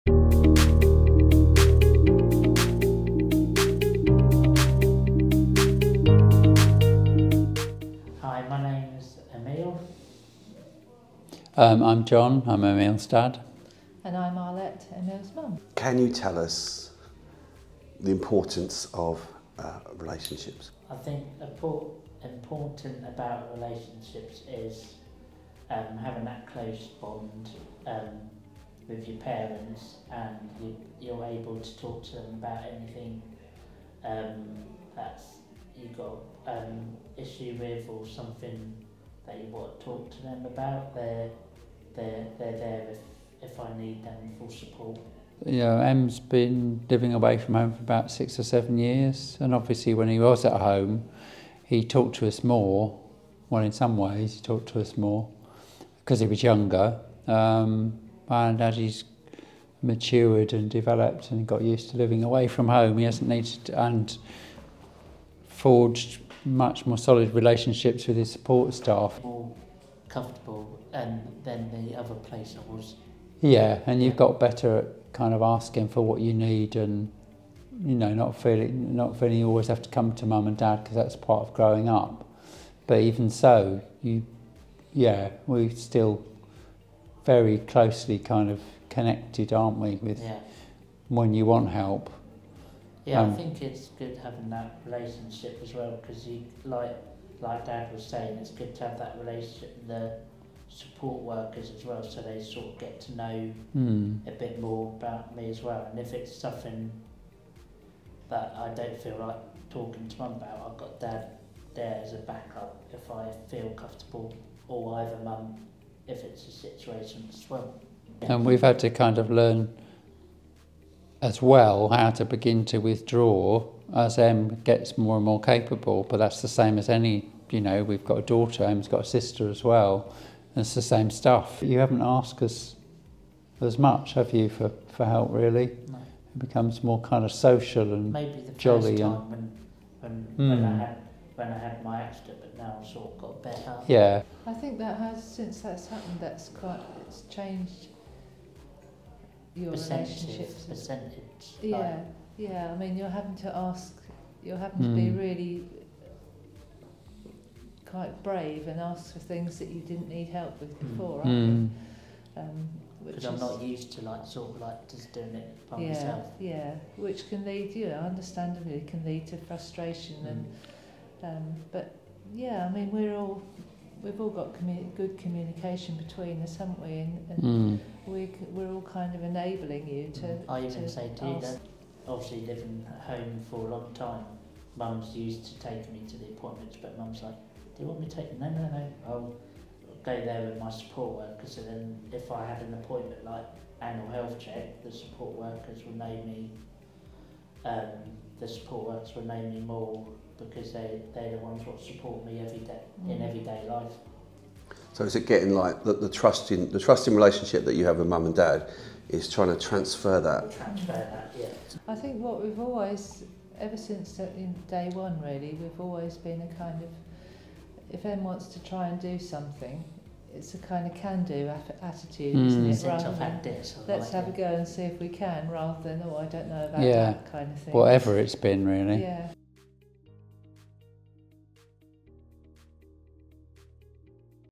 Emile and his parents talk about their relationship
In this video, Emile and his parents talk about their positive relationship towards each other.